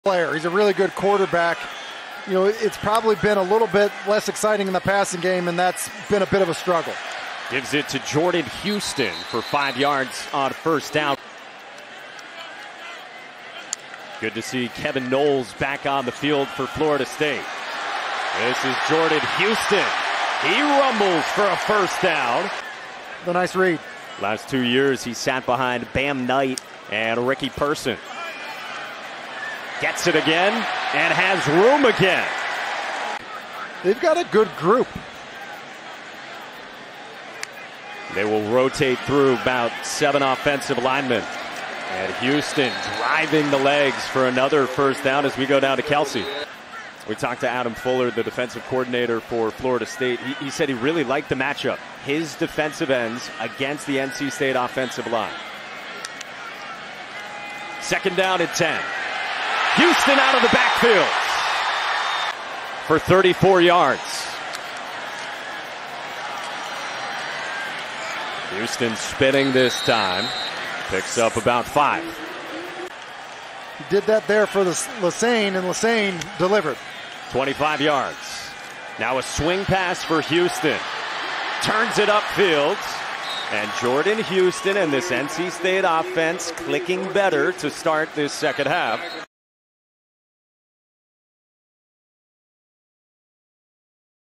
NC State RB Jordan Houston Highlights vs. Florida St.
With quarterback Devin Leary and running back Demie Sumo-Karngbaye both on the sideline in the second half due to injuries, running back Jordan Houston became the go-to play on the NC State offense. Houston ran the ball 24 times for 90 yards and caught two passes for 41 yards in the comeback victory for NC State.